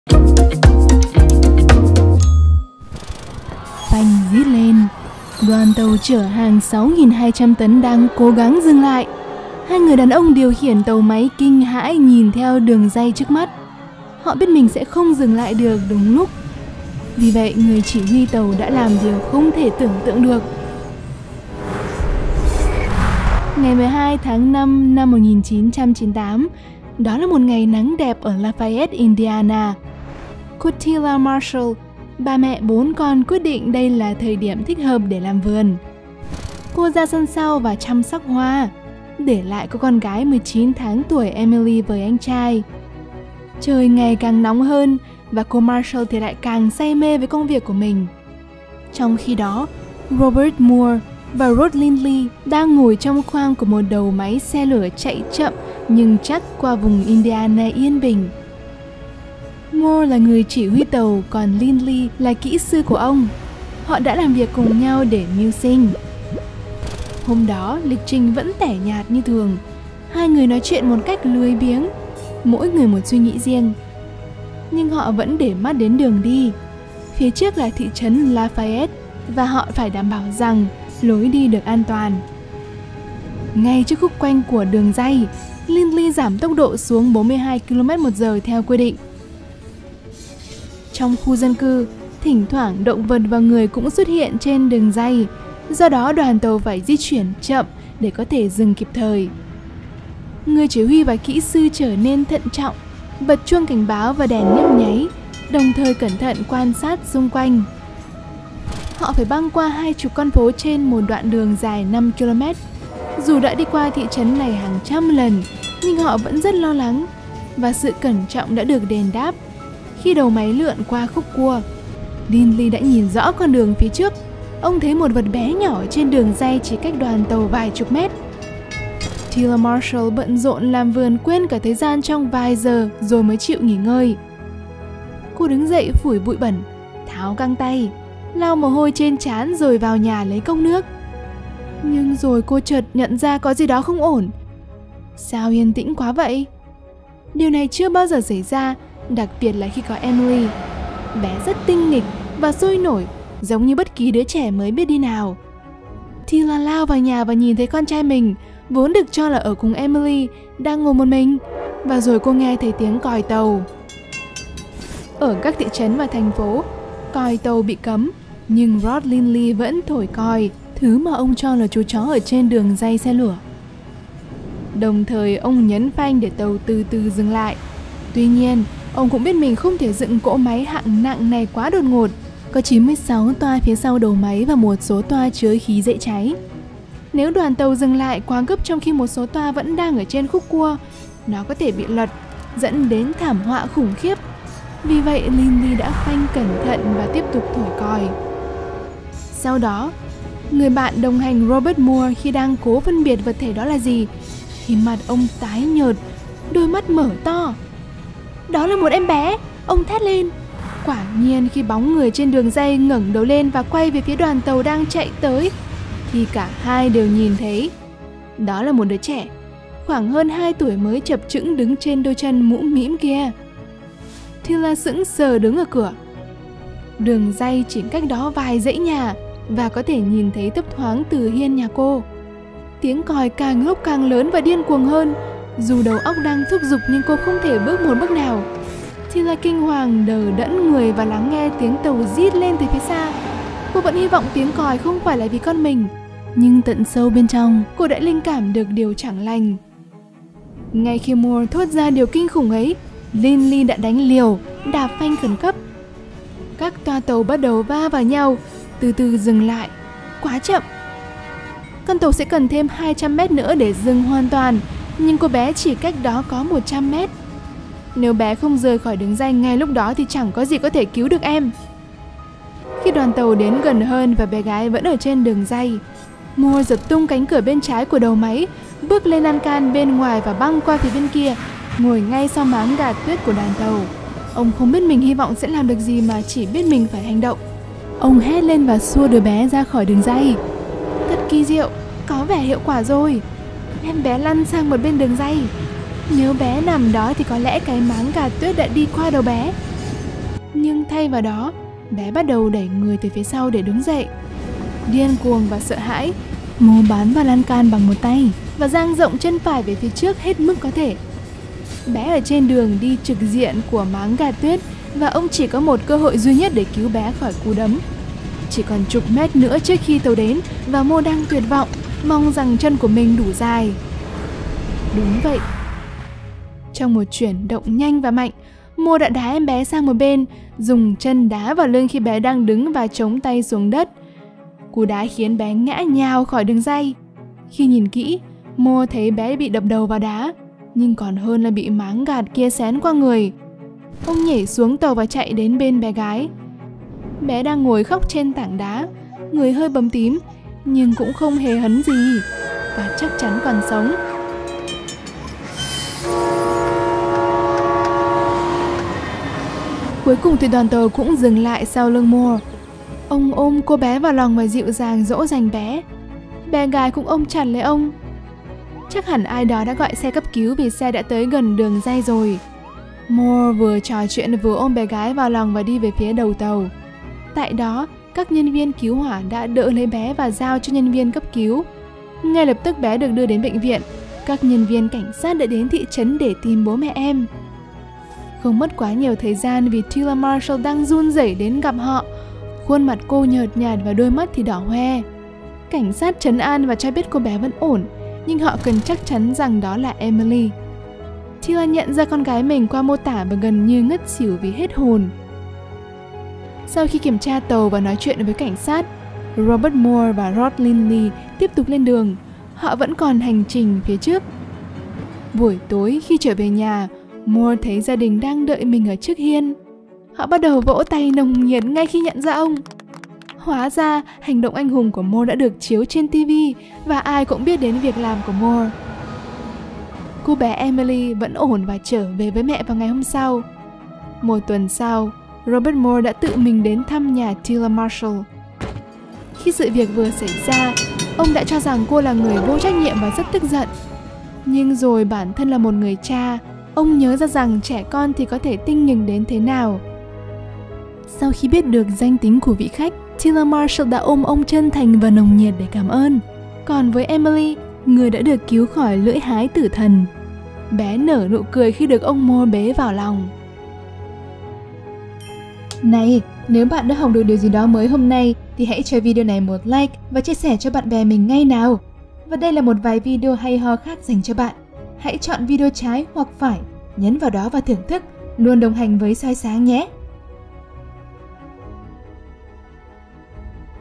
Em Bé Ở Trên Đường Ray Khi Tàu Hỏa Lao Tới: Câu Chuyện Anh Hùng
Xin chào! Bạn có thích những câu chuyện sinh tồn có thật không? Hãy xem video này nhé! Tượng tưởng này – phanh rít lên, đoàn tàu chở hàng 6.200 tấn đang cố gắng dừng lại. Hai người đàn ông điều khiển tàu máy kinh hãi nhìn theo đường ray trước mặt. Họ biết mình sẽ không dừng lại được đúng lúc. Vì vậy, người chỉ huy tàu đã làm điều không thể tưởng tượng được…
Đó là một ngày nắng đẹp ở Lafayette, Indiana, cô Tila Marshall, bà mẹ bốn con quyết định đây là thời điểm thích hợp để làm vườn . Cô ra sân sau và chăm sóc hoa, để lại cô con gái 19 tháng tuổi Emily với anh trai. Trong khi đó, Robert Mohr và Rod Lindley đang ngồi trong khoang của một đầu máy xe lửa chạy chậm nhưng chắc qua vùng Indiana yên bình. Ngay khi tàu máy vòng ở khúc quanh, Lindley thấy một thứ gì đó nhỏ chỉ cách tàu vài chục mét.